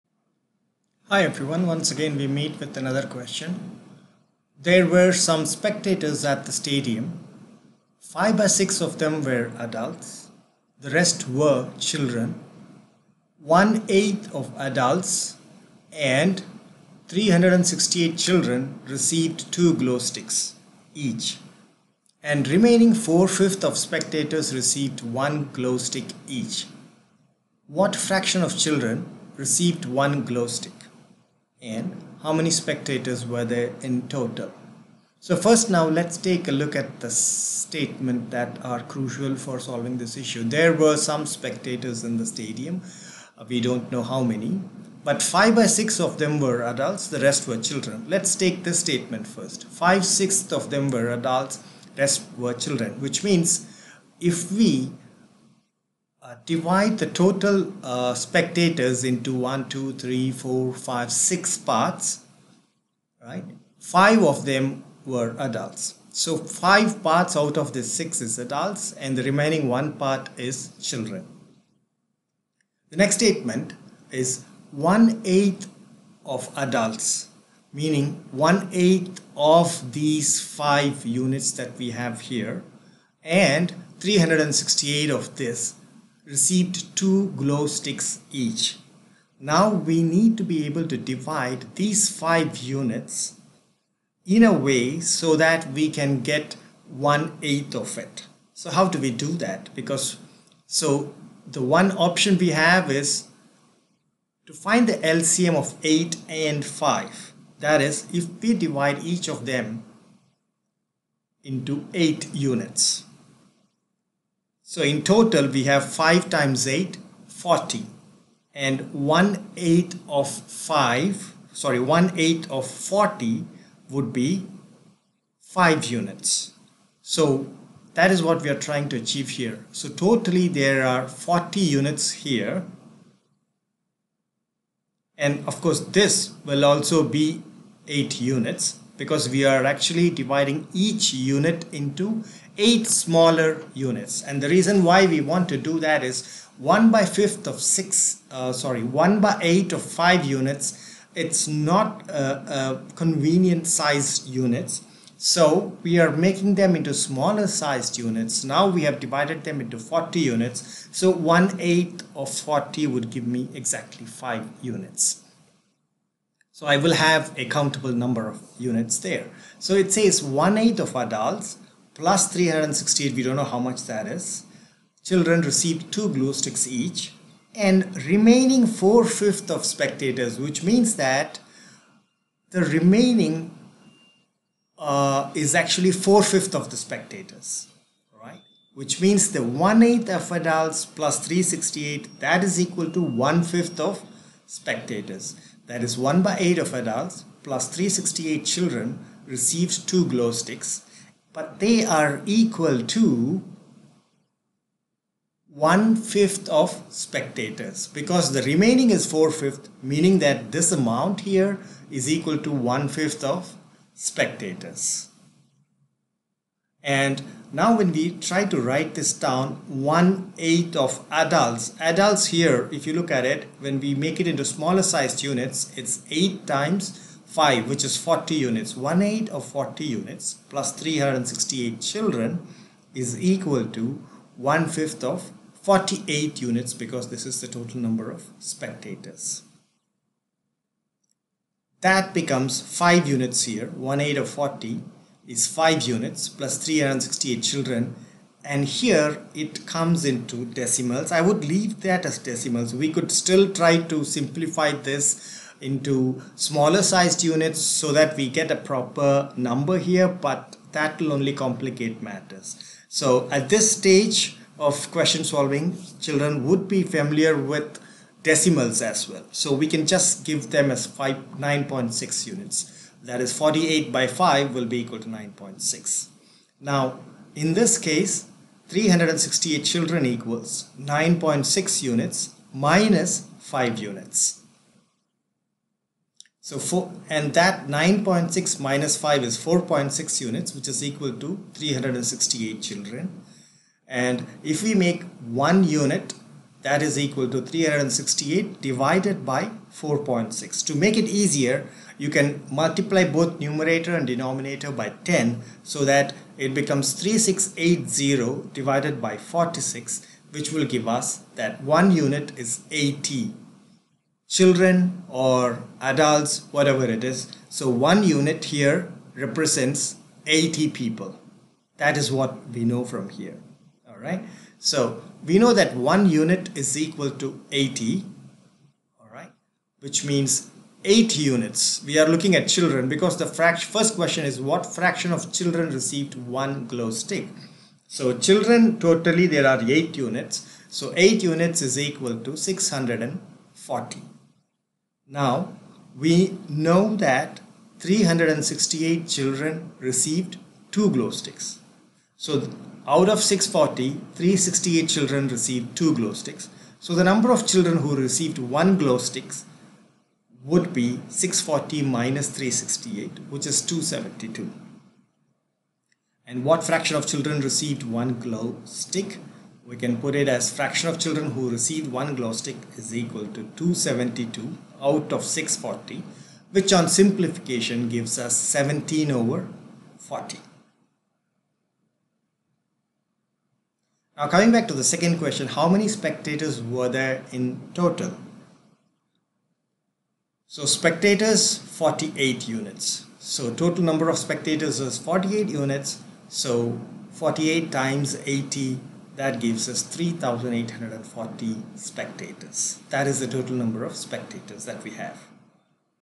Singapore Math - Primary 5/ Primary 6 Fractions
We use Singapore Math to solve the following question.

There were some spectators at the stadium. 5/6 of them were Adults, rest were children. 1/8 of Adults & 368 children received 2 glow sticks each & remaining 4/5 of spectators received 1 Glow stick each .
A) what fraction of children received 1 Glow stick ?
B) How many spectators were there in TOTAL ?

This question is apt for grade 5/grade 6.

Do check out my YouTube channel for more Singapore Math related videos